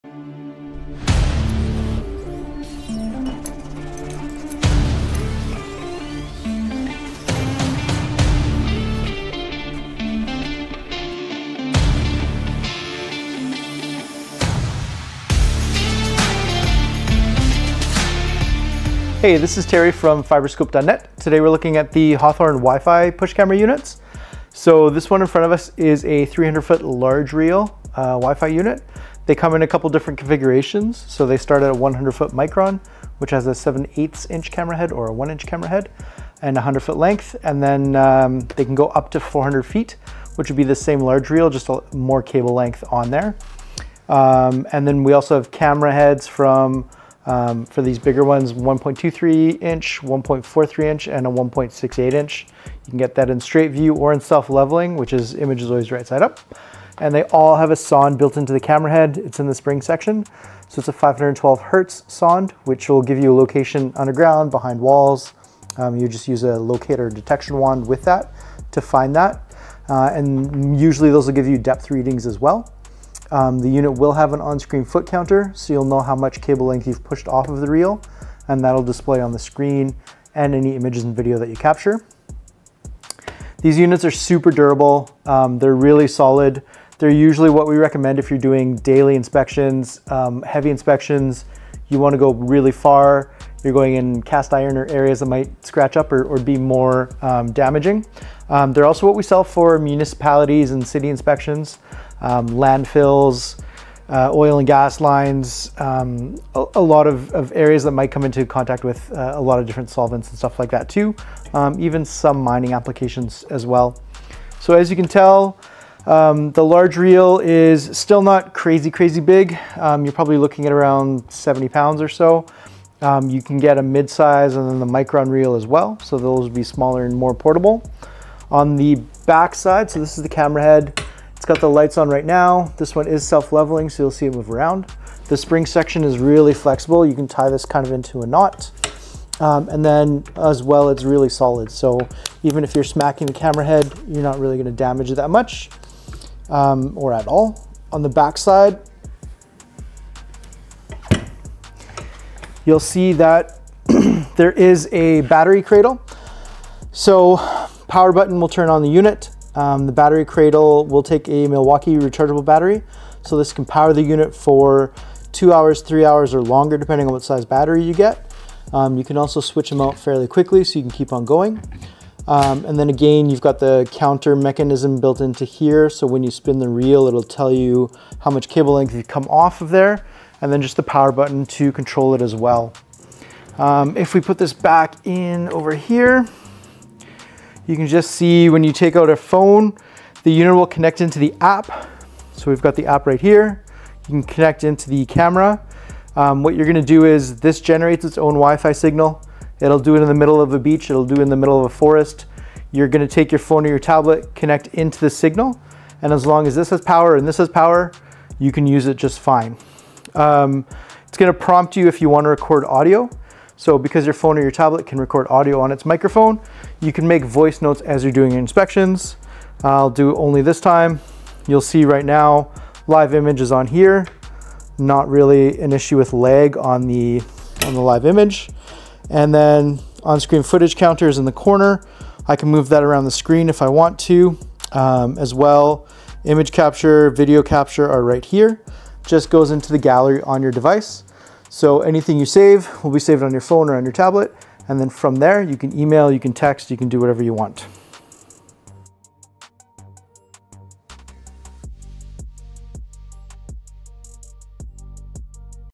Hathorn Wifi SEWER CAMERA INTRO Overview
Hathorn Wifi Sewer Camera Unit

- Designed and Manufactured in North America: Built for demanding industry applications.
- Robust Construction: Butt-welded steel frame and stainless steel camera heads ensure durability.
- Customizable Push Rod Sizes: Allows for pushing up to 400 feet while maintaining field reliability.
- Seamless Video Streaming: Streams video to Apple or Android devices via the PipeSTREAM™ app.
- User-Friendly App: Take screenshots, overlay voice commentary, and share videos with up to four devices.
- Versatile Camera Heads:
7/8" Micro Camera Head: Suitable for pipes up to 2" diameter.
1.68" Camera Head: Suitable for pipes up to 12" diameter.
- Built-In LED Lights: Enhances visibility in all camera heads.
IP68 Waterproof Rating: Ensures the camera heads are fully waterproof.

00:00 Trailer
00:19 Introduction
01:24 Durability and Applications
02:29 Battery & Wifi Design
03:05 Wifi Connection